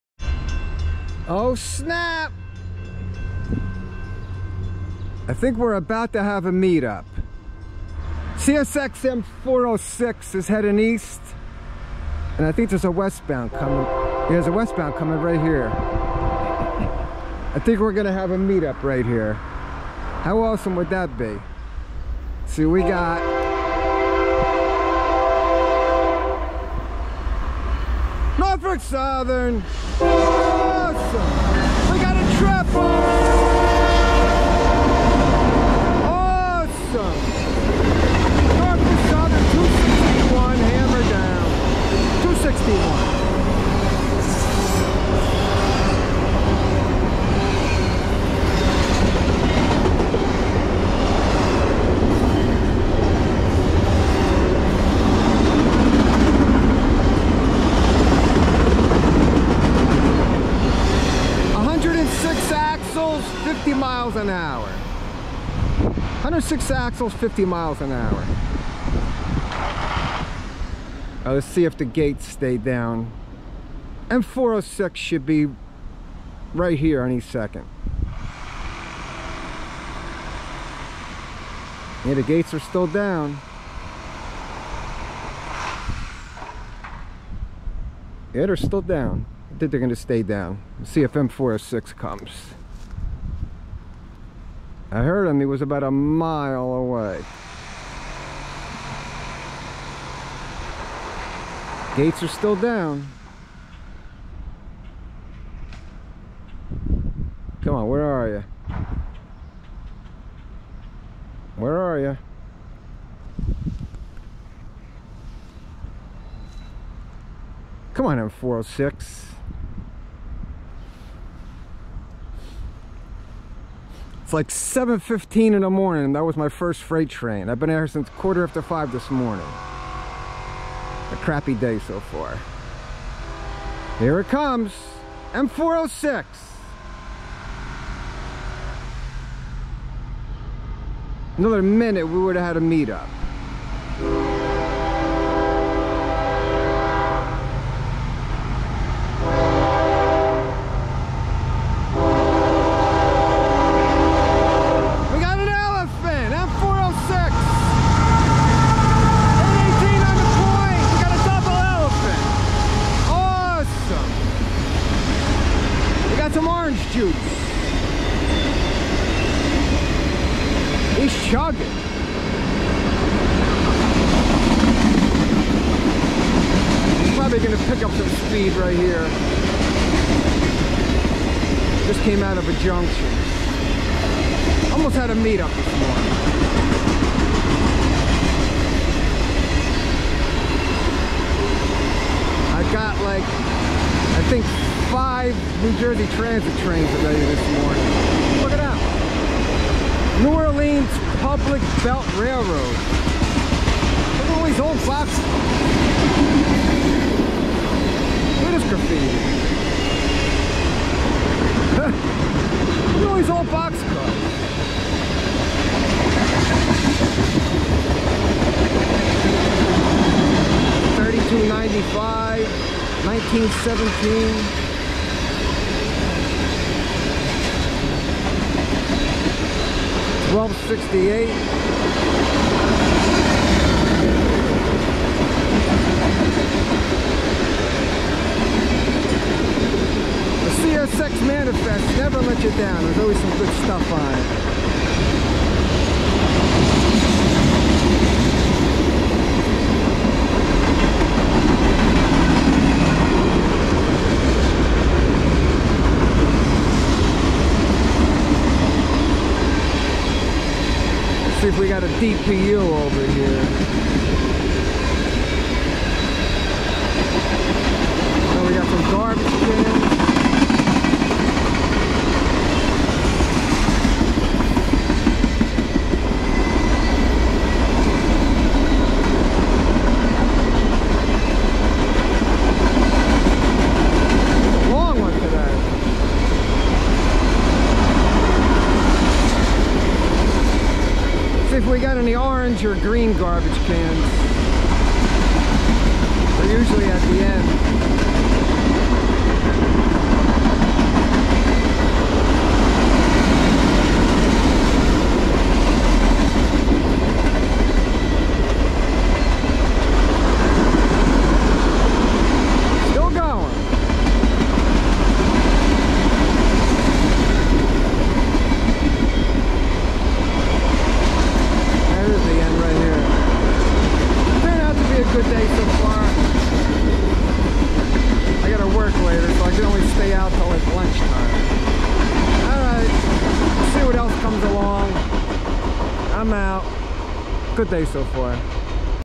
[4K] 06/20/2023 CSX & Norfolksouthern Meetup catch missed by a hot minute! @Middlesex, NJ
Almost had a meetup catch! NS train 261 ripping Westbound at 50 MPH with 106 Axles- 7637 ES44DC leading C40-9W 9559 and EMD SD70ACe #1110 . A minute later CSX Manifest train M406 comes through the same crossing with elephant style ES44AC-H 818 leading ET44AH 3403 .